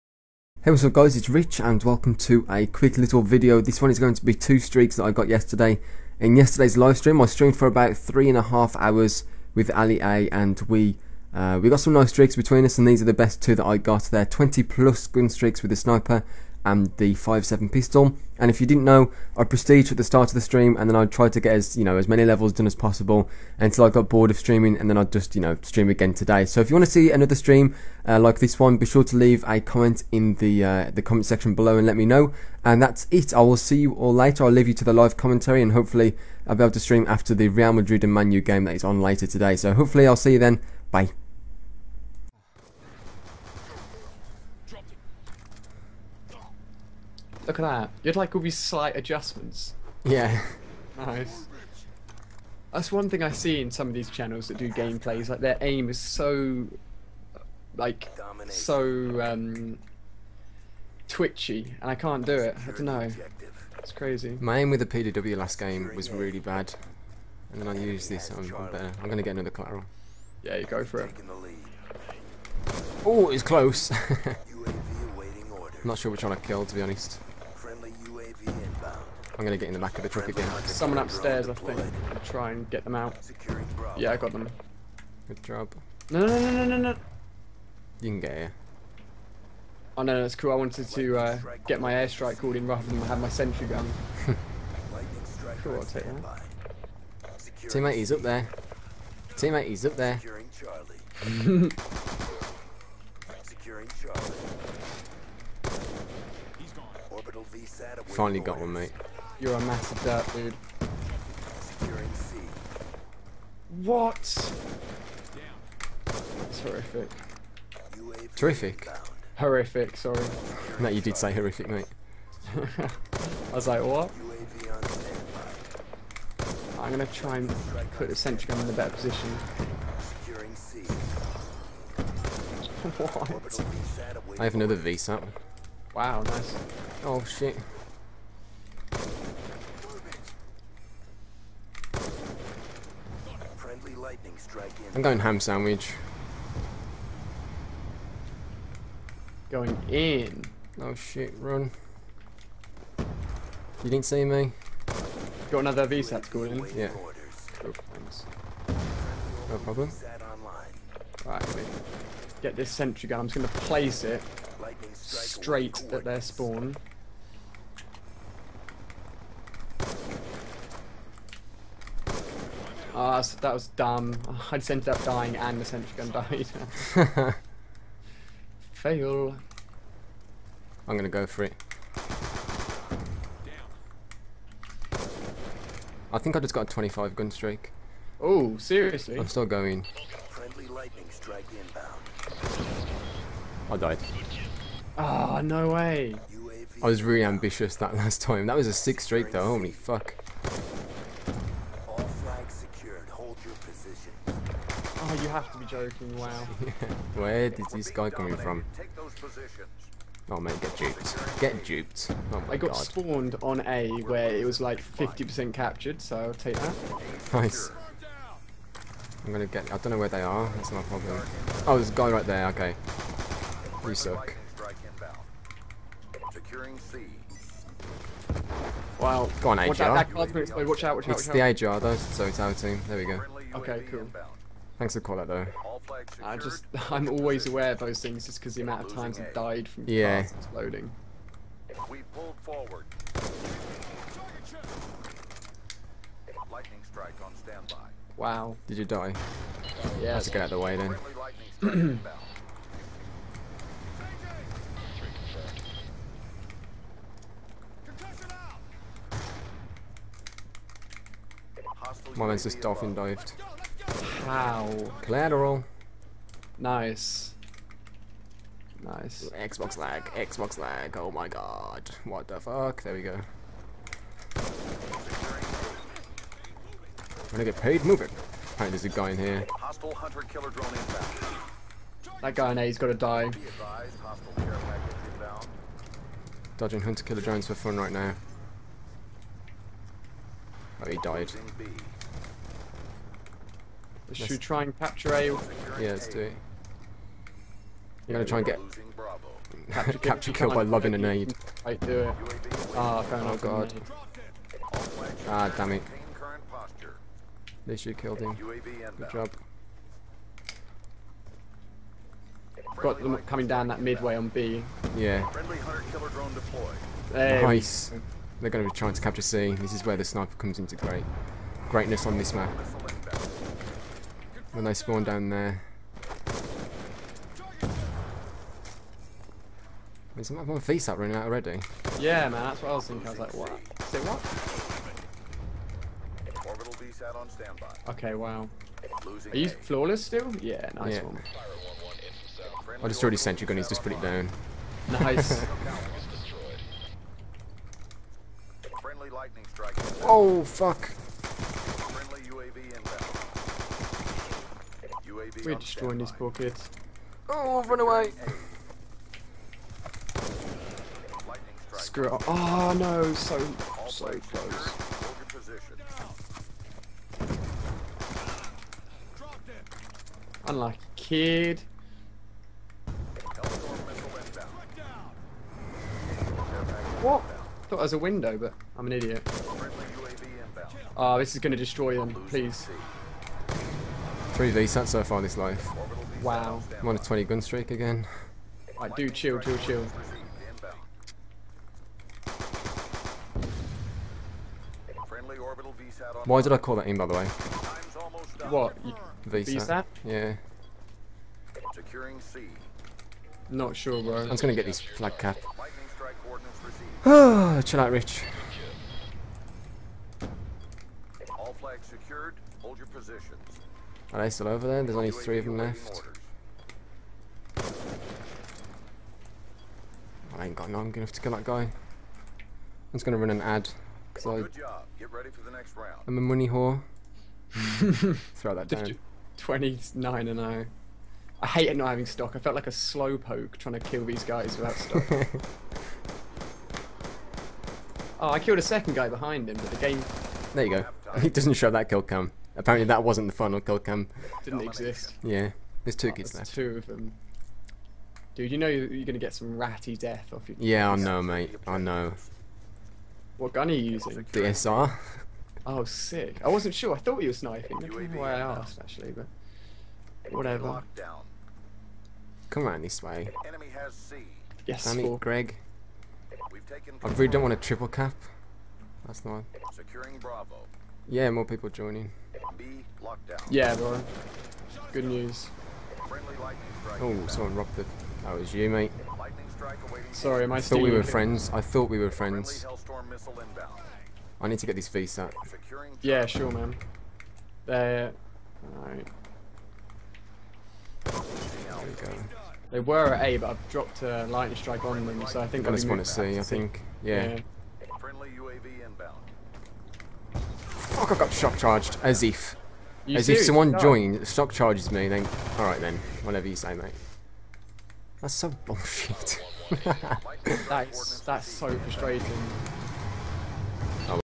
Two Epic 20+ Sniping Gunstreaks! (Diamond DSR Sniper and Diamond Five-Seven Pistol Gameplay)
Can we hit 1500 likes please? Thanks!

Send in your top 5 clips to:
iTemptop5cod ( @ ) hotmail.co.uk (no spaces or brackets)

iTemp.